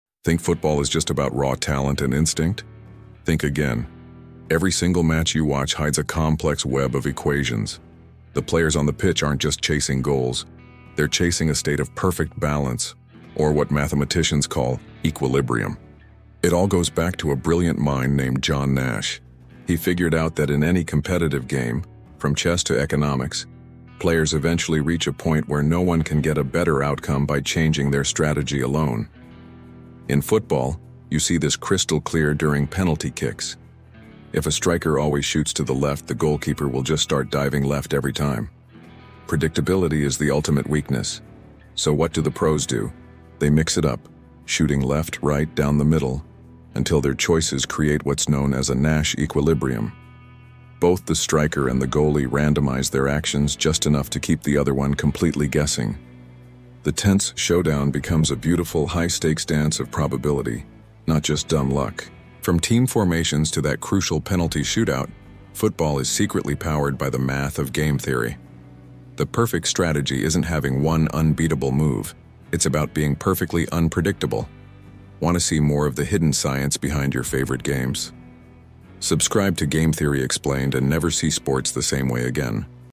Hidden Strategies in Football Nash on the Field
Football isn’t chaos—it’s equilibrium. Discover how John Nash’s theory explains penalty kicks, formations, and why perfect strategy means being unpredictable.